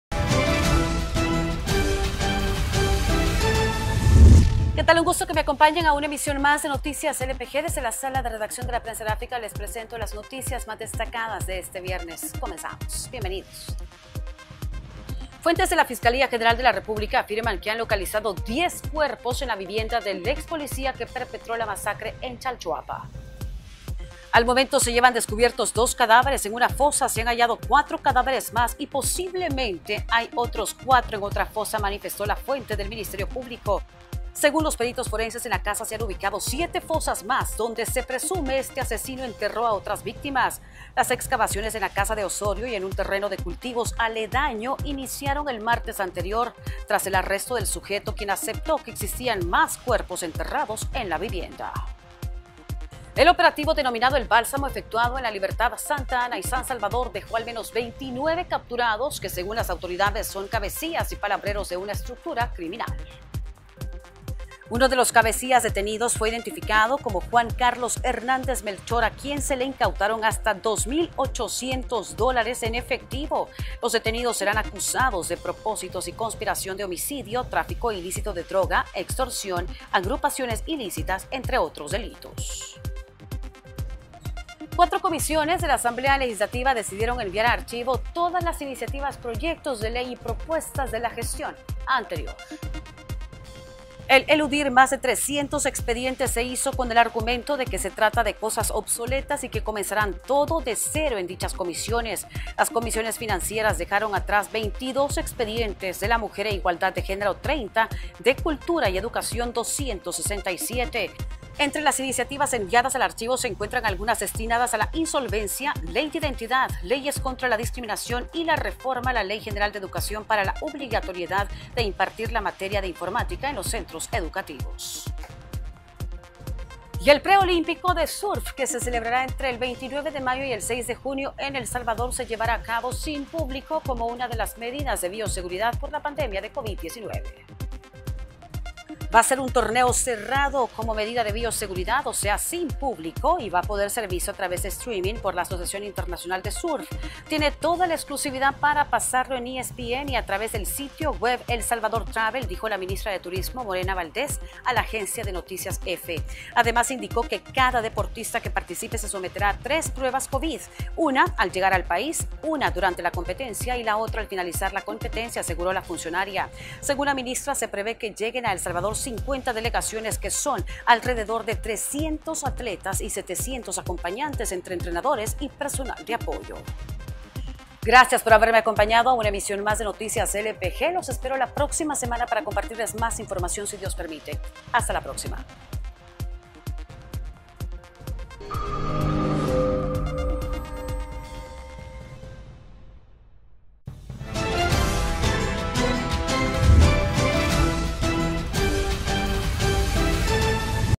Noticiero LPG 14 mayo: Diez cadáveres encontrados en vivienda del expolicía acusado de masacre
¡Visítenos!